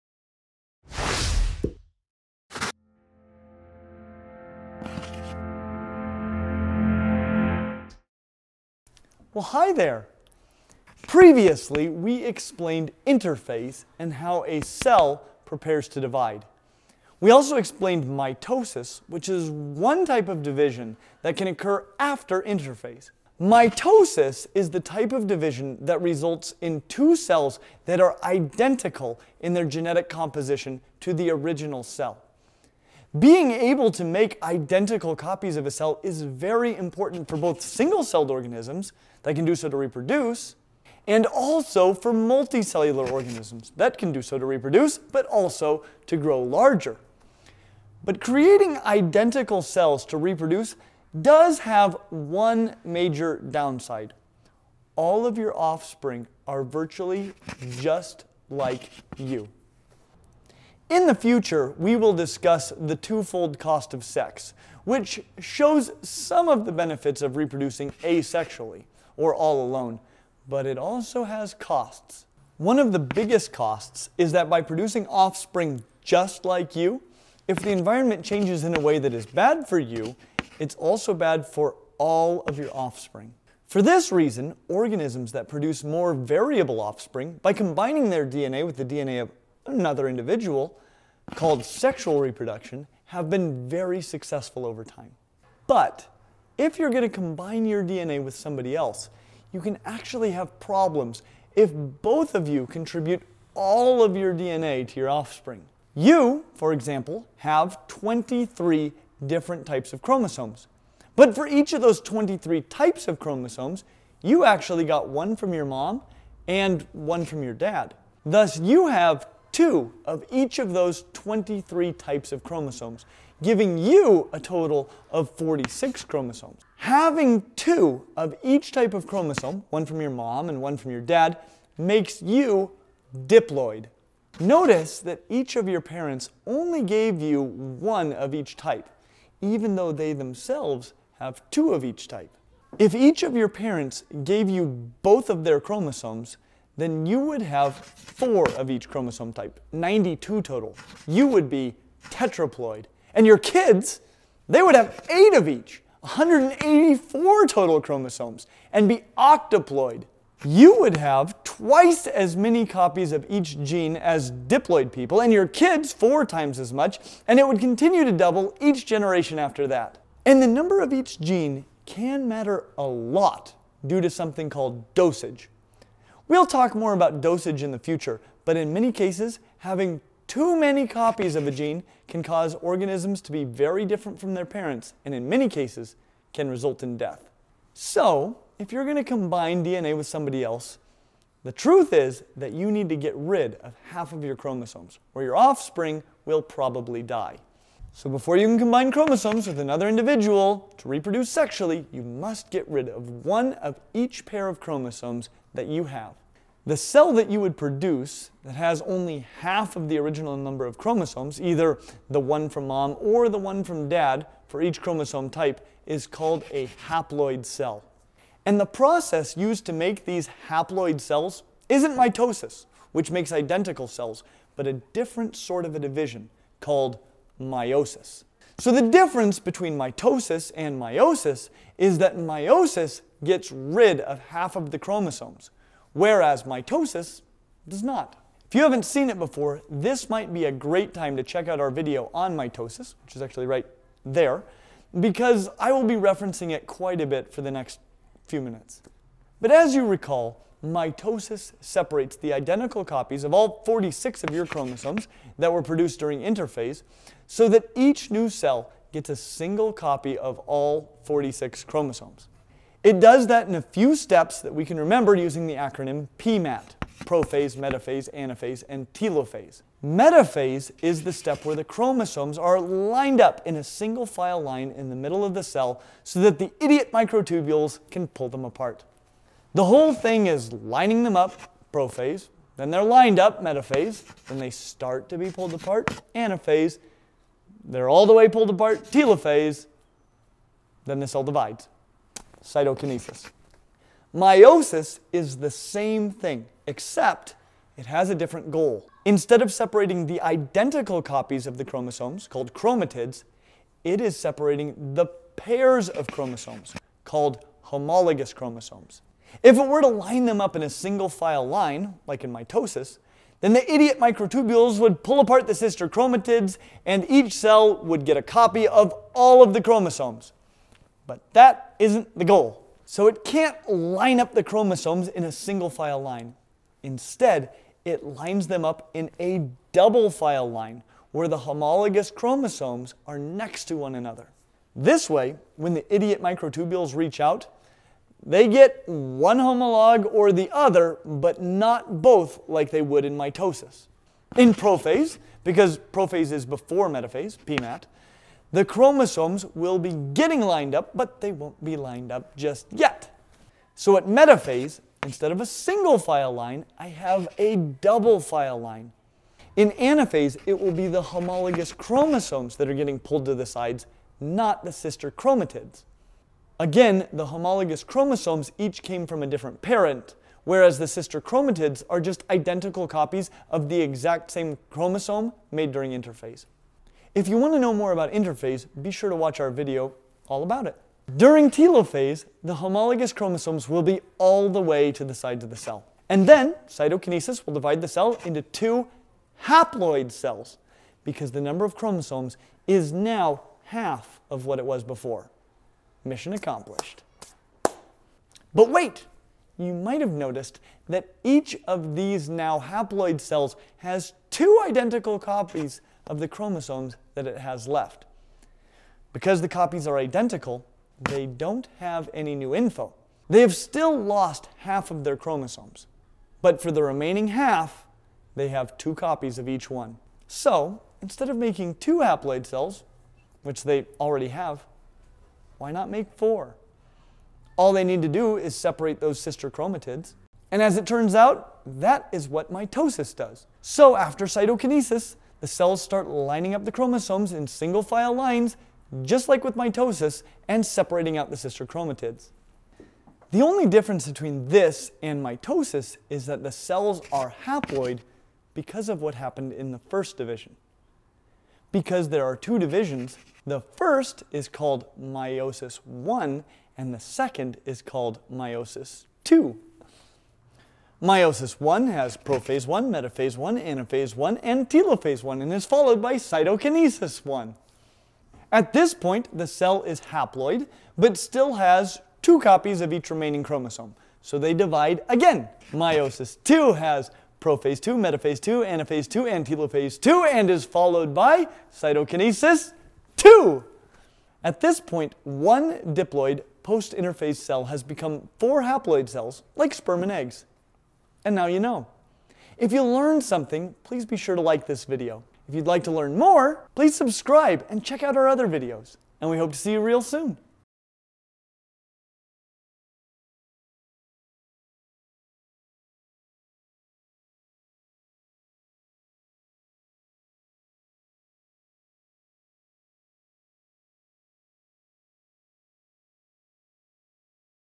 Clint Explains Meiosis - Just Like Mitosis (Except Completely Different)
Mitosis is great at making exact copies of cells, but when organisms reproduce sexually, making a copy of all of your chromosomes means that your babies end up with way too many chromosomes. So how do you get rid of some? Meiosis, that's how! Unfortunately, it still has to be run by those idiot microtubules, so how do they pull this off? Clint will explain!